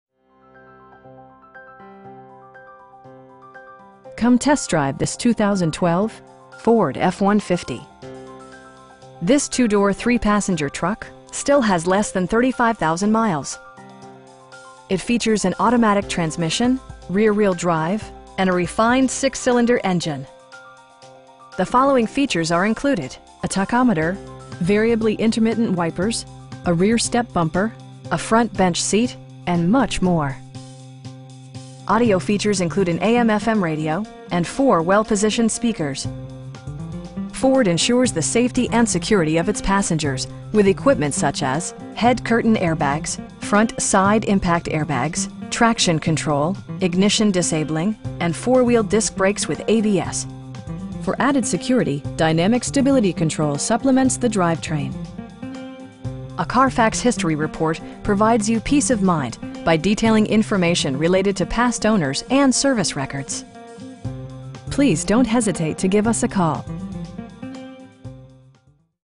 2012 Ford F-150 in Houston, TX 77090
Munday Chevrolet
17800 North Fwy

Come test drive this 2012 Ford F-150. This 2 door, 3 passenger truck still has less than 35,000 miles! It features an automatic transmission, rear-wheel drive, and a refined 6 cylinder engine. The following features are included: a tachometer, variably intermittent wipers, a rear step bumper, a front bench seat, air conditioning, tilt steering wheel, and much more. Audio features include an AM/FM radio, and 4 speakers, providing excellent sound throughout the cabin. Ford ensures the safety and security of its passengers with equipment such as: head curtain airbags, front SIDE impact airbags, traction control, ignition disabling, and 4 wheel disc brakes with ABS. For added security, dynamic Stability Control supplements the drivetrain. A Carfax history report provides you peace of mind by detailing information related to past owners and service records. We know that you have high expectations, and we enjoy the challenge of meeting and exceeding them! Please don't hesitate to give us a call.